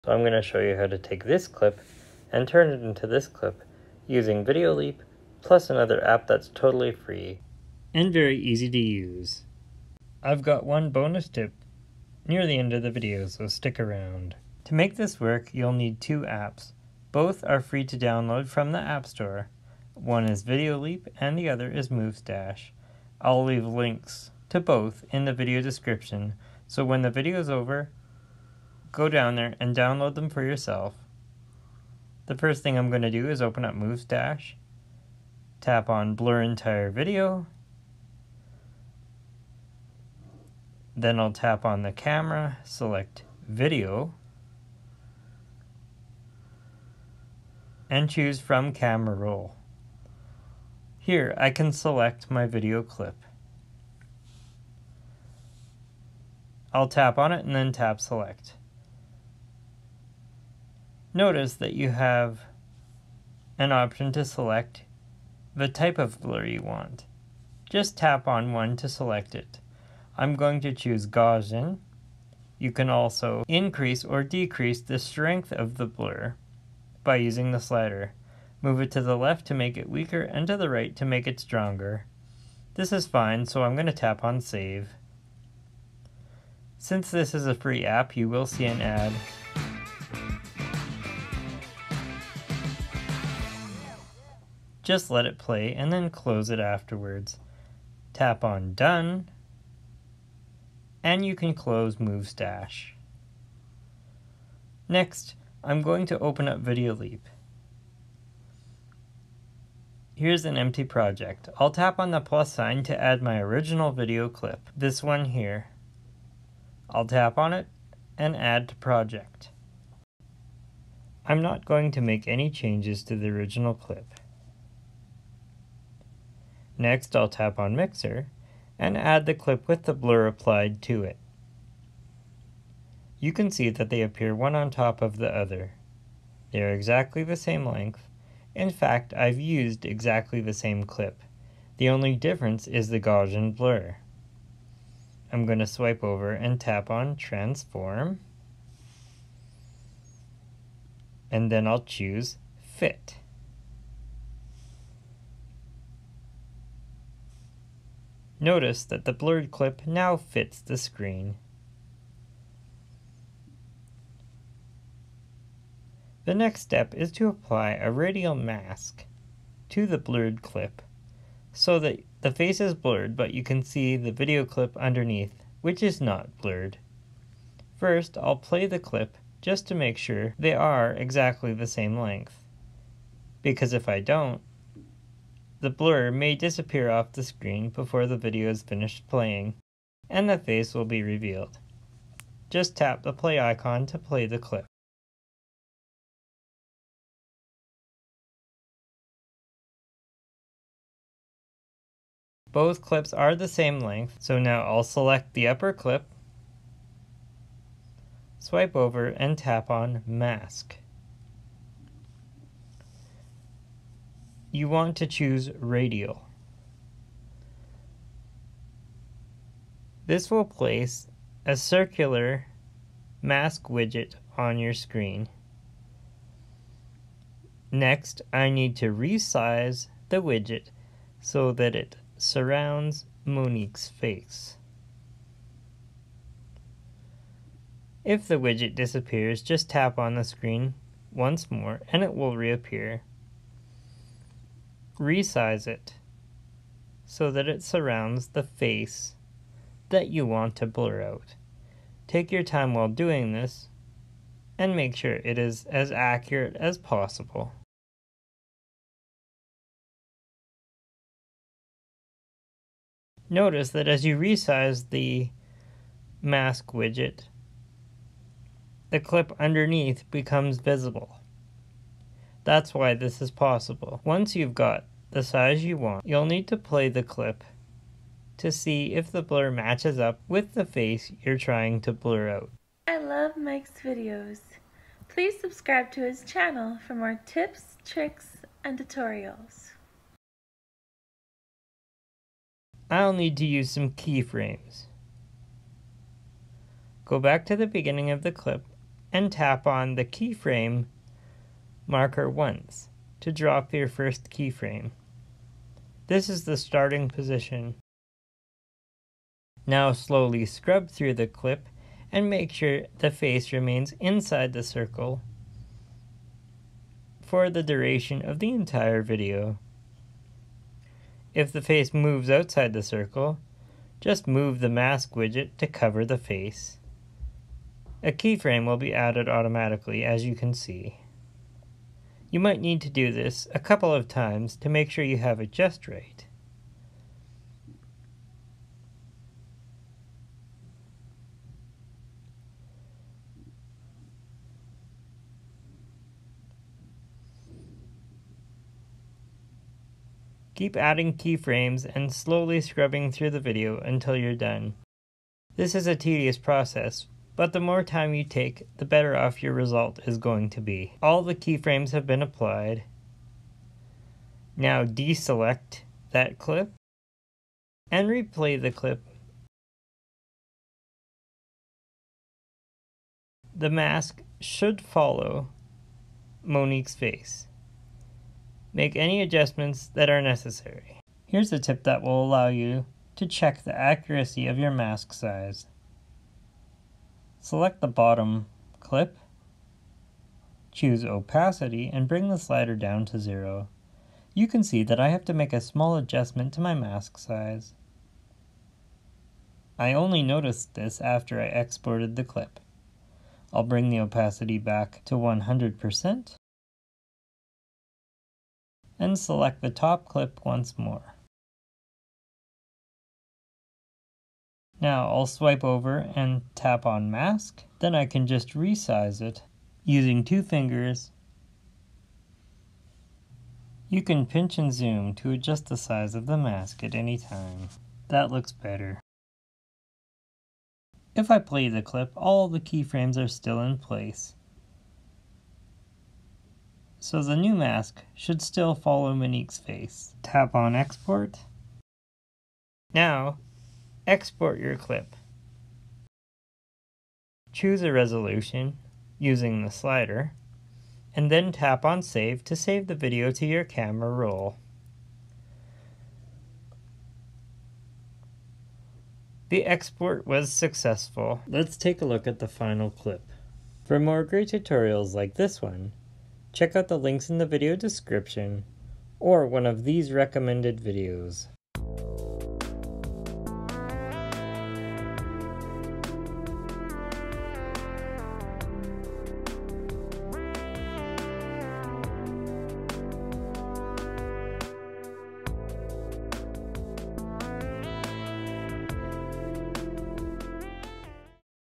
How To Blur Faces In Video| Videoleap Tutorial
I show you how to blur faces in YouTube videos using a Gaussian blur videoleap. There is no gaussian blur available in the Videoleap app. Videoleap uses a pixelate filter to blur out objects and faces. I don’t like the way it looks. So I researched this topic and came up with this method. Stick around for the entire video so you don’t miss my bonus tip.

Check out some of my favorite creators here